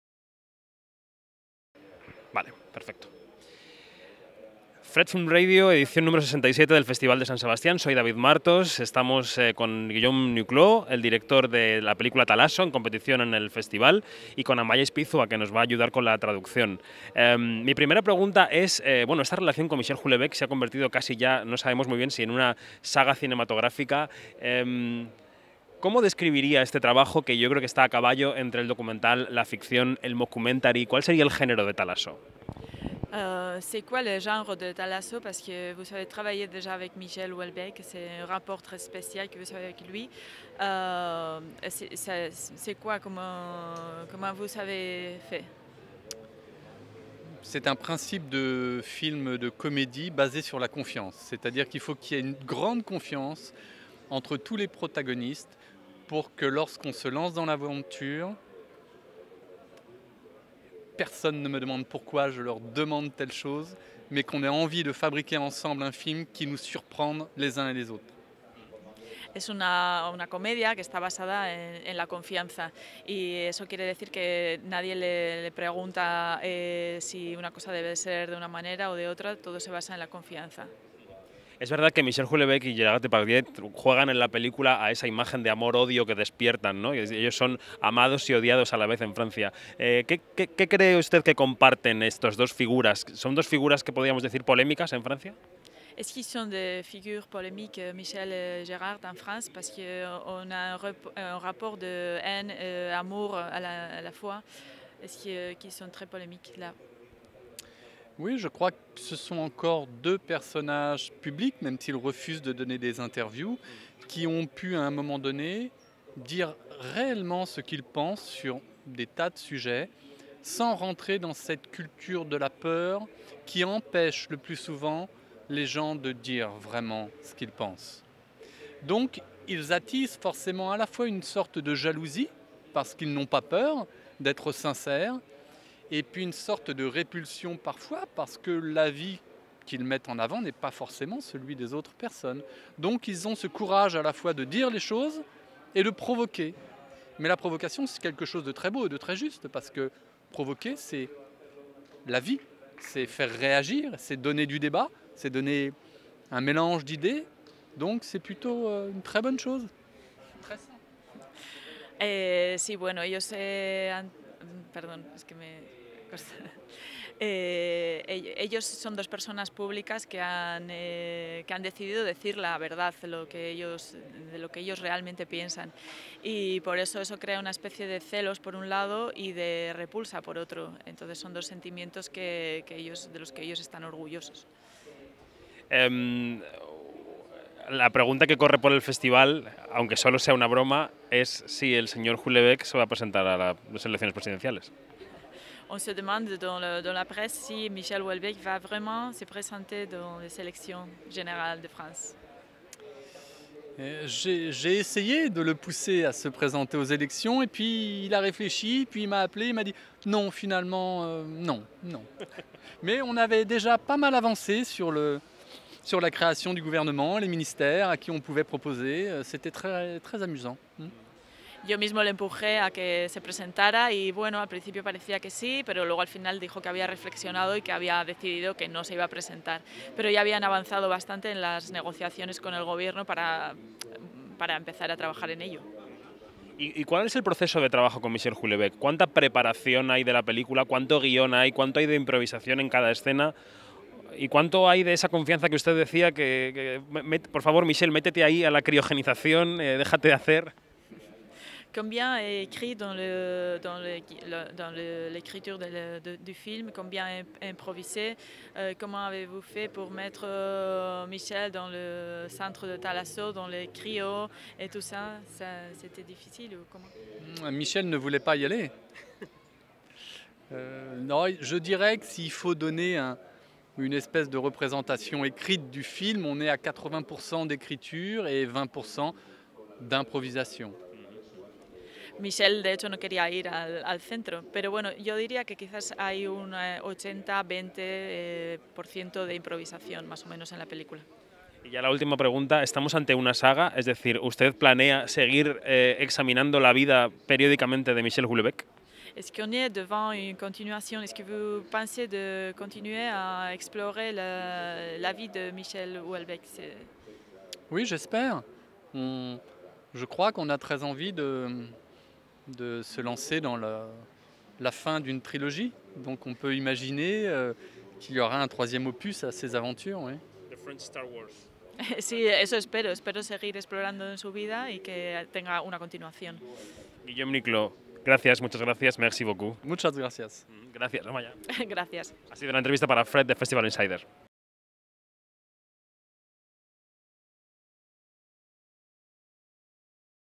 Guillaume Nicloux - THALASSO - 67 San Sebastian International Film Festival
GuillaumeNicloux presents his new film THALASSO at FredFilmRadio during the 67SSIFF.
Interview by David Martos.